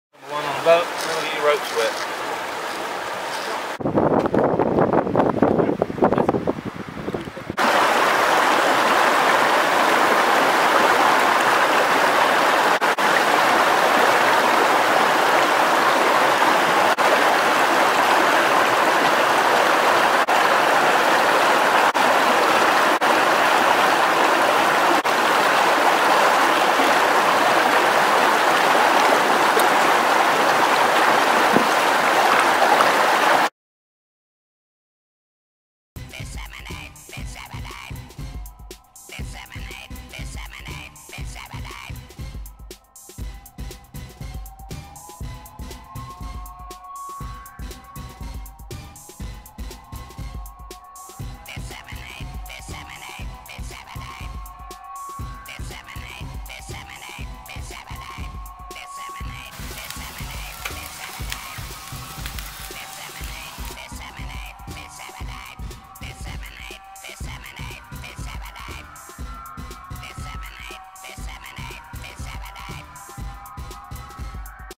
Silvers, Only On Light Duty's Today, Just Out For A Trot, To Make A Drop & Pick Up At A Neighbours.
All in a days work on Al's Farm AKA Art Retreat Bakrit, More Later!